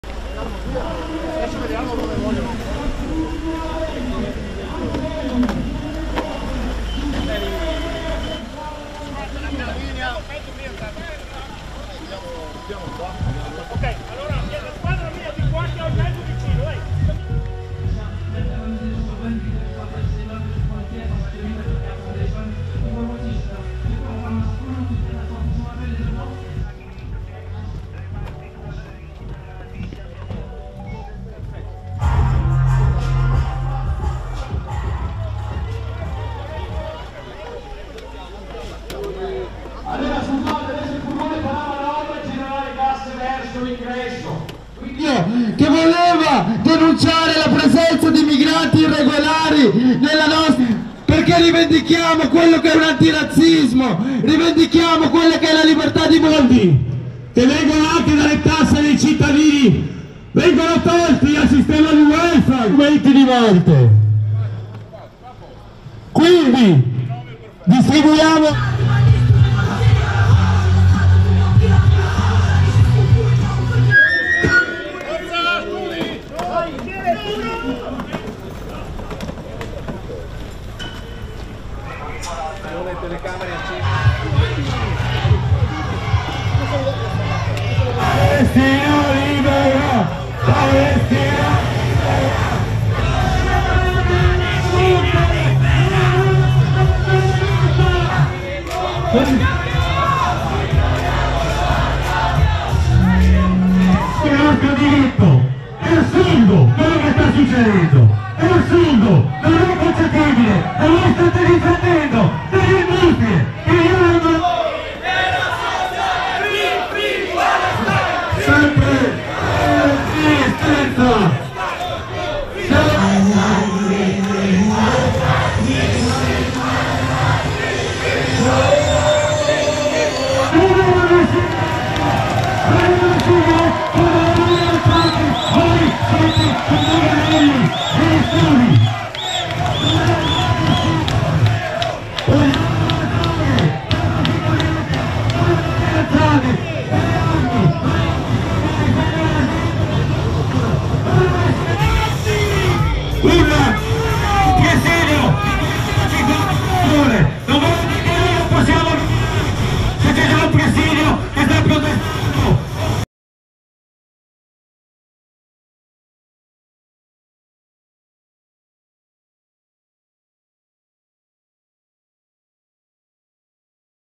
Verona, imponente schieramento di mezzi e uomini dei reparti mobili della polizia
Numerose camionette dei reparti mobili della polizia sbarrano la strada e si posizionano per impedire il passaggio dei manifestanti scesi in piazza per protestare contro la fiera delle armi.